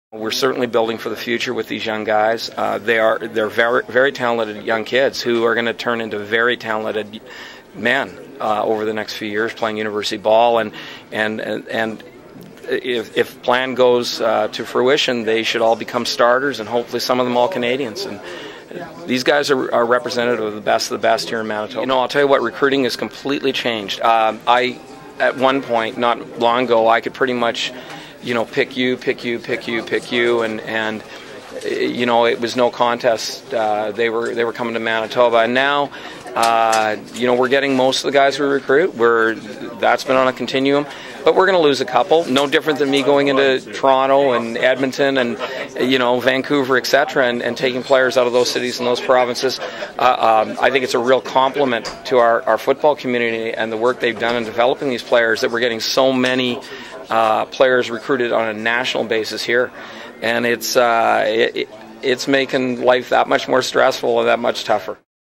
U of M Bisons - New Football Recruits - March 30, 2010 - Winnipeg, Manitoba
Manitoba Bison football head coach Brian Dobie introduces five new recruits for the 2010 season on Tuesday, March 30, 2010.